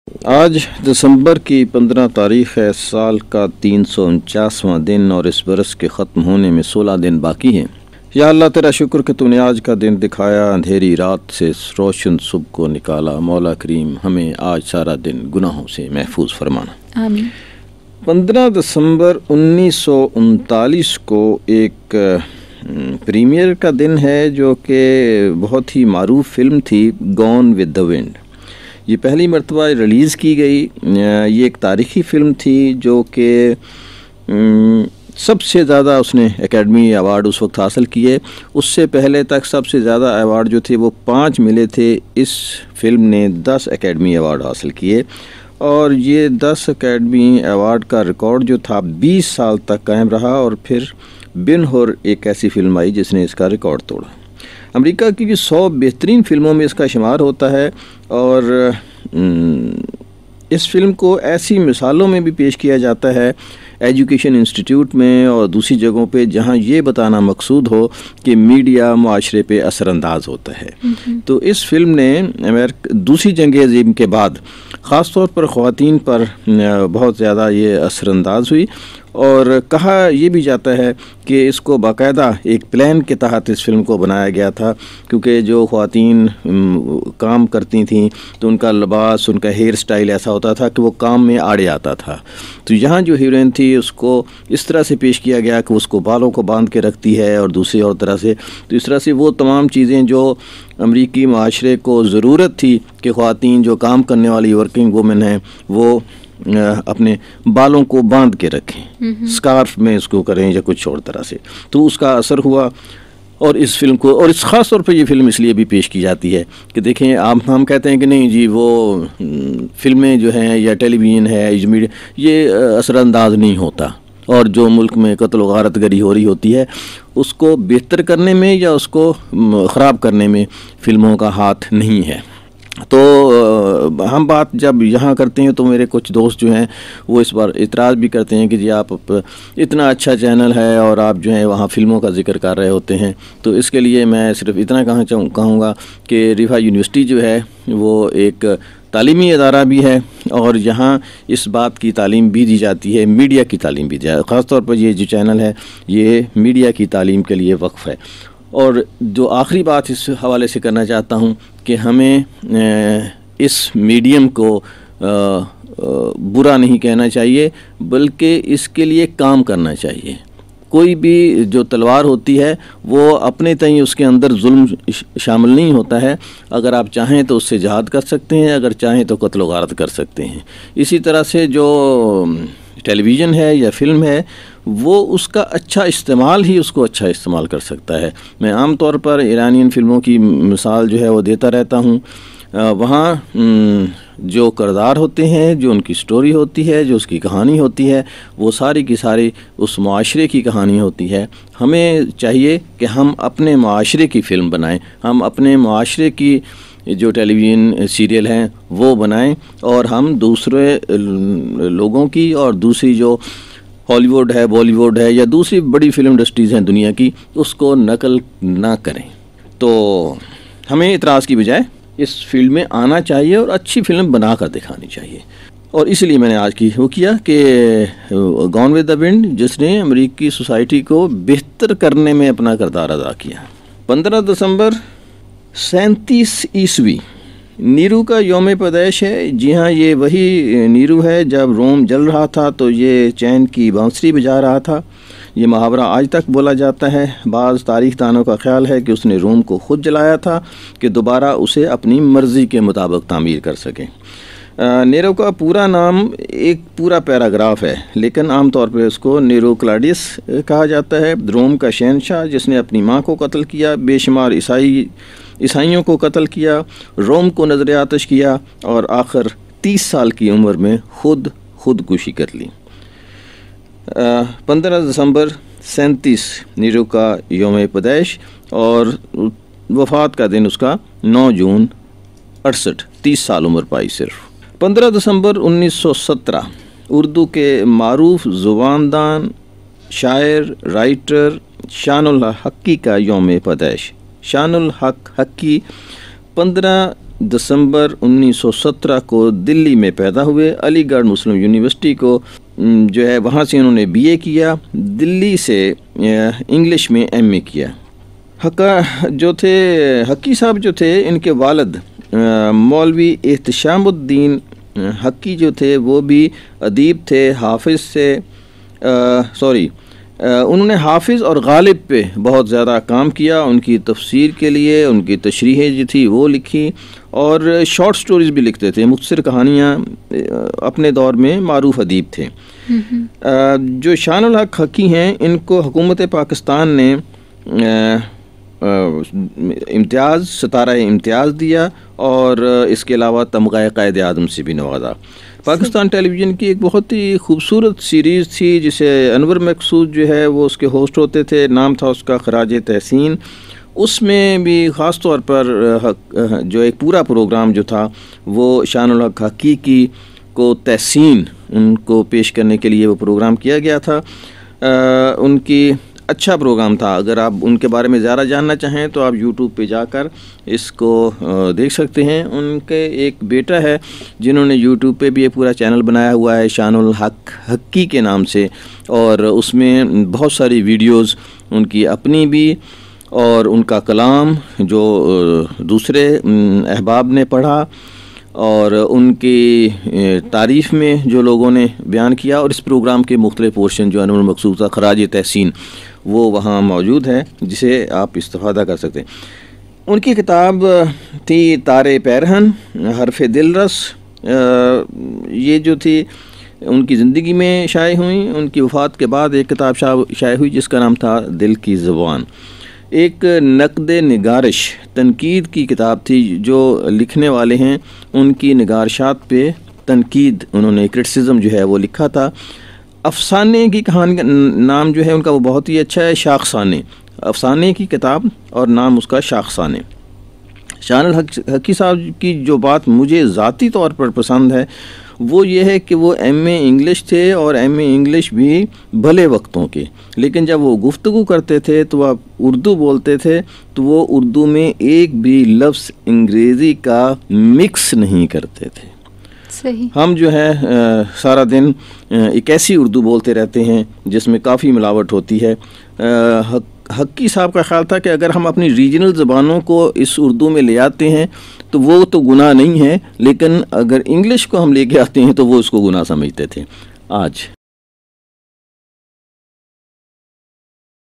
AAJ Today in History December - 15
1-1939 – Gone with the Wind (highest inflation adjusted grossing film) receives its premiere at Loew's Grand Theatre in Atlanta, Georgia, United States.
2-AD 37 – Nero, Roman emperor (d. 68)
3-1917 – Shan-ul-Haq Haqqee, Indian-Pakistani linguist and lexicographer (d. 2005)
Voice and written by Azhar Niaz
Producer Azhar Niaz
Riphah FM 102.2
Riphah international University Pakistan